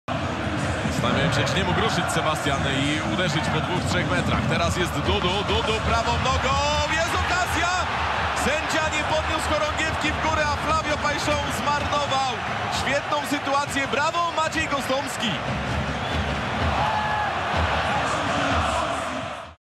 Maciej Gostomski - Najlepsza Interwencja 14 kolejki T-ME
Najlepsza interwencja 14 kolejki T-ME.

W niej interwencja Macieja Gostomskiego w meczu ze Śląskiem Wrocław.